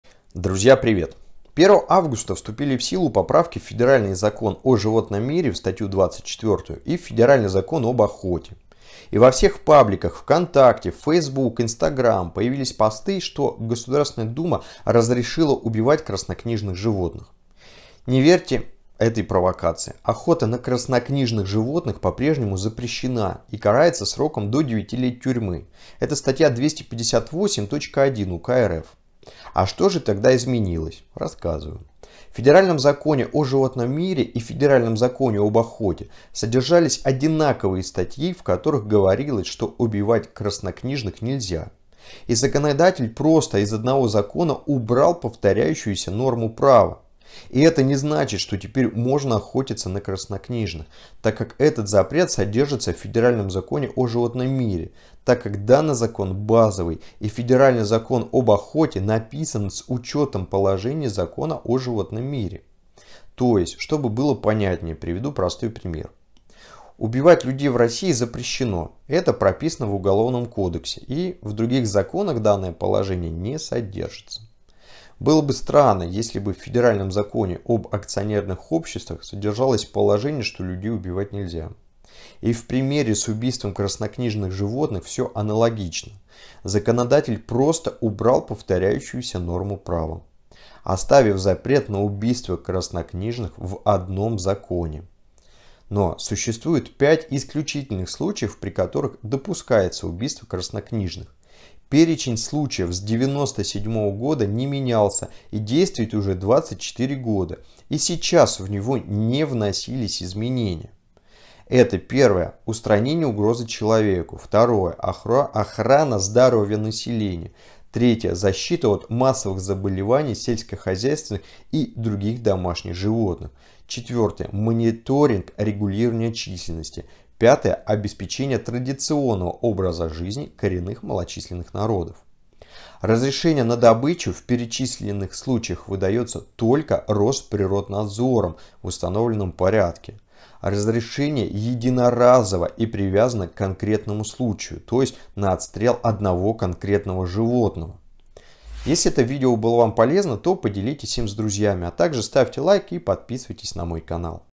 Государственная Дума разрешила убивать краснокнижных животных. Комментарии юриста
Друзья привет! 1 августа вступили в силу поправки в федеральные законы «О животном мире» (статья 24) и «Об охоте». И во всех пабликах Вконтакте, фейсбука, инстаграмм появились посты, что государственная дума разрешила убивать краснокнижных животных.
Не верьте этим провакациям, охота на краснокнижных животных по прежнему запрещена и карается сроком до 9 лет тюрьмы (ст. 258.1 УК РФ).
А что же тогда изменилось? Рассказываю.
В Федеральном законе «О животном мире» и в федеральном законе «Об охоте» содержались одинаковые статьи, в которых говорилось, что убивать краснокнижных нельзя. И законодатель просто из одного закона убрал посторяющуюся норму права. Но, это не значит, что теперь можно охотиться на краснокнижных, т.к. этот запрет содержится в федеральном законе «о животном мире», т.к. данный закон базовый и федеральный закон «об охоте» написан с учетом положений закона о животном мире.
⠀Т.е. чтобы было понятнее, приведу пример. Убивать людей в России запрещено. Это прописано в уголовном кодексе, и в других законах данное положение не содержится.
Было бы странно, если бы в федеральном законе «Об акционерных обществах» содержалось положение, что людей убивать нельзя.
И в примере с убийством краснокнижных животных все аналогично. Законодатель просто убрал повторяющуюся норму права. Оставив запрет на убийство краснокнижных в одном законе.
Но, существует пять исключительных случаев, при которых допускается убийство краснокнижных. Перечень случаев с 1997 года не менялся и действует уже 24 года. И сейчас в него не вносились изменения. Это
1. устранение угрозы человеку ⠀
2. охрана здоровья населения,
3. защита от массовых заболеваний сельскохозяйственных и других домашних животных,
4. мониторинг и регулирование численности,
5. обеспечение традиционного образа жизни коренных малочисленных народов.
Разрешение на добычу в перечисленных случаях выдается только Росприроднадзором в установленном порядке.  Разрешение единоразово и привязано к конкретному случаю.

00:00 Введение
00:18 Объяснение
01:35 Исключительные случаи на отстрел краснокнижных животных
02:28 Заключение